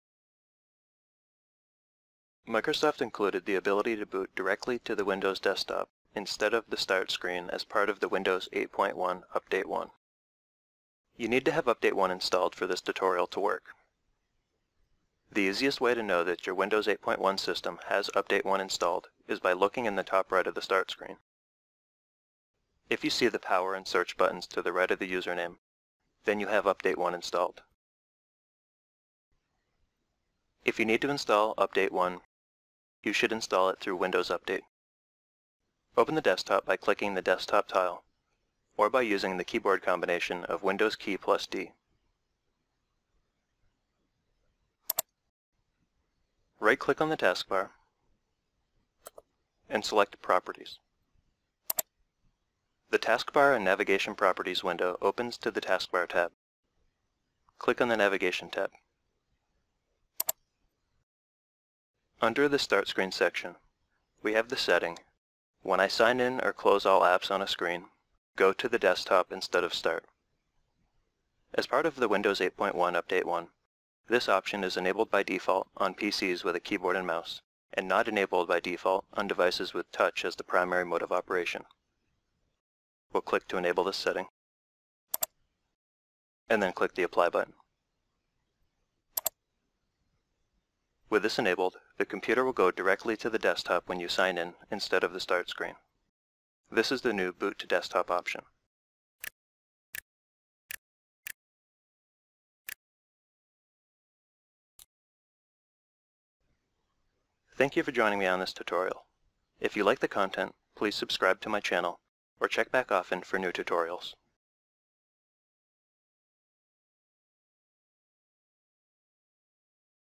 Windows 8.1 - Boot To Desktop [Tutorial]
This tutorial covers how to set a Windows 8.1 PC to boot to the Windows desktop instead of the Start Screen.  You need to have Windows 8.1 with Update 1 installed for this to work.